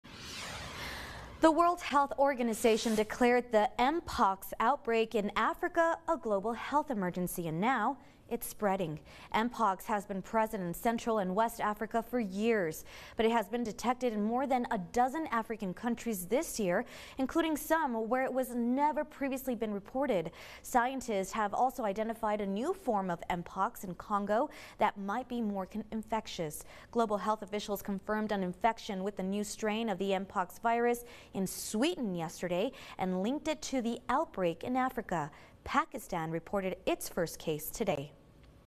WHO declares MPOX outbreak a global health emergency
The World Health Organization is announcing the MPOX outbreak in Africa as a global health emergency.